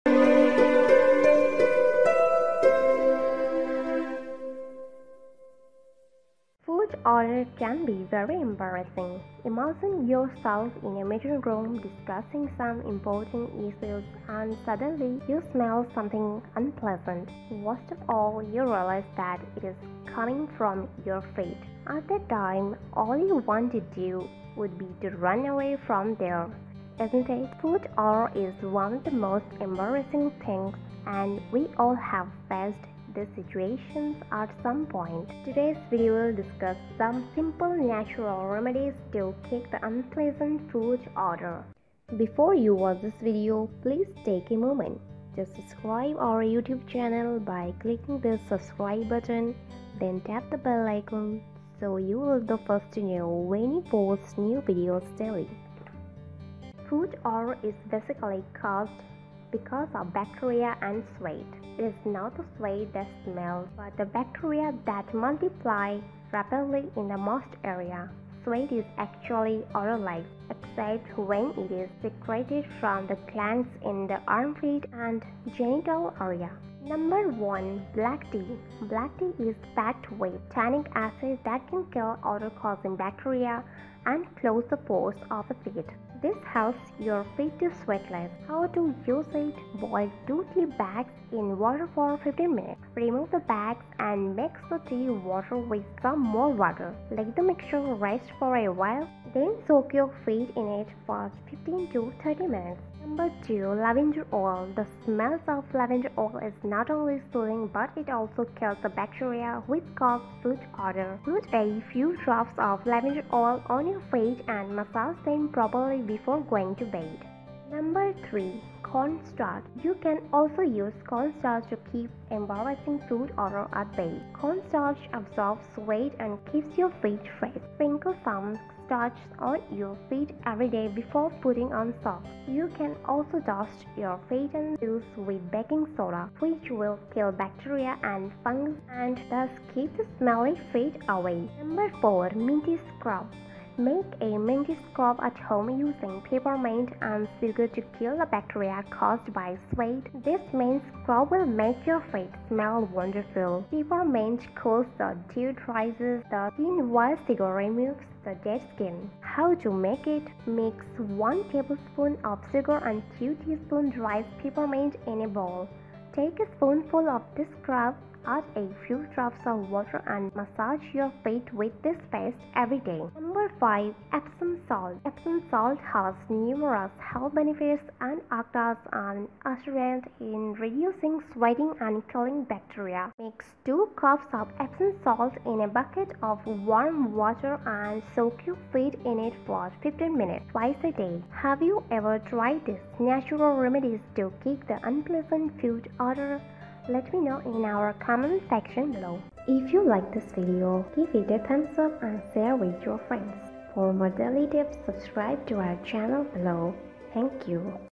How To Get Rid Of Foot Odor Fast And Naturally | Get Rid Of Foot Odour With These Home Ingredients
Foot smell can be exceptionally humiliating.

Envision yourself in a gathering room talking about some significant issues and abruptly you smell something horrendous. To top it all off, you understand that it is originating from your feet. Around then, all you need to do is fled from that point. Is it accurate to say that it isn't? Foot smell is a standout amongst the most humiliating things and we as a whole have confronted this circumstance sooner or later.

The present video will talk about some basic common solutions for kick the unsavory foot scent.

Foot smell is fundamentally caused as a result of microorganisms and sweat. It isn't the perspiration that smells, yet the microorganisms that duplicate quickly in the soggy territory. Sweat is really scentless, with the exception of when it is emitted from the organs in the armpit and genital region.

1) Black tea

Dark tea is pressed with tannic acids that can execute smell causing microscopic organisms and close the pores of the feet. This encourages your feet to sweat less.

Step by step instructions to utilize it: Boil two tea packs in water for 15 minutes. Evacuate the sacks and blend the tea water with some more water. Give the blend a chance to rest for some time, at that point absorb your feet it for 15-30 minutes.

2) Lavender oil

The smell of lavender oil isn't just relieving however it additionally slaughters the microorganisms, which cause foot scent. Put a couple of drops of lavender oil on your feet and back rub them appropriately before hitting the hay.

Have you ever attempt these regular solutions for kick the horrendous foot scent, Let me know in our remark area underneath.